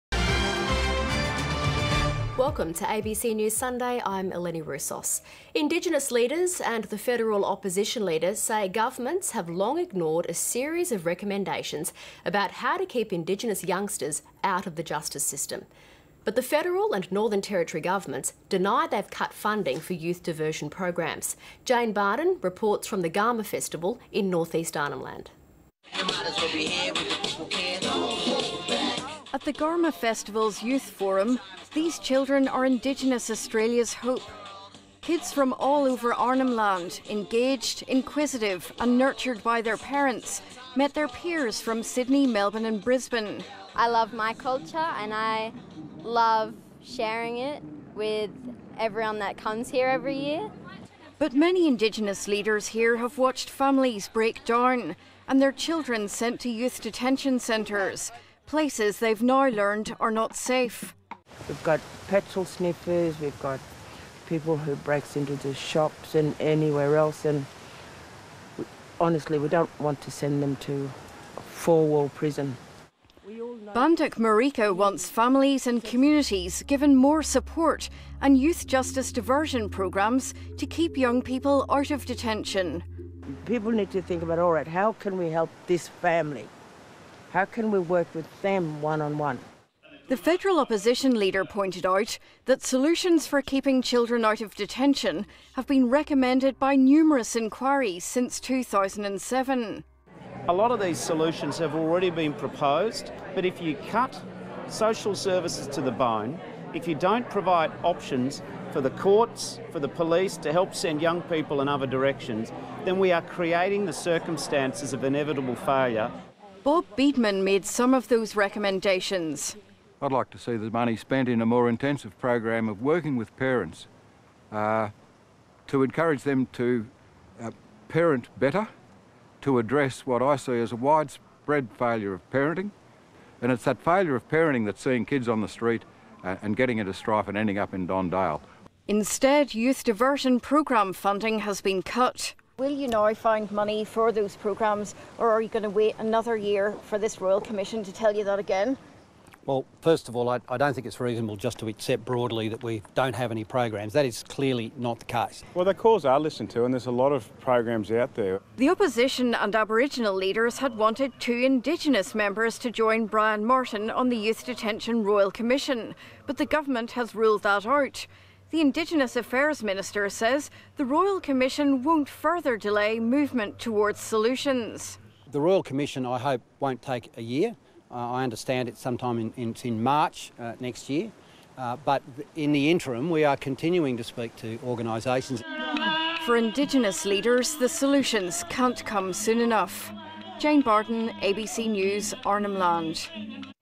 Dismay over prospects for another delay to investment in NT youth diversion programs
Indigenous leaders and the Federal Opposition leader have told the Garma festival in Arnhem Land that outrage over allegations of mistreatment of juveniles in detention have been compounded by cuts to youth diversion and justice reinvestment programs.
31/07/2016, Jane Bardon, ABC News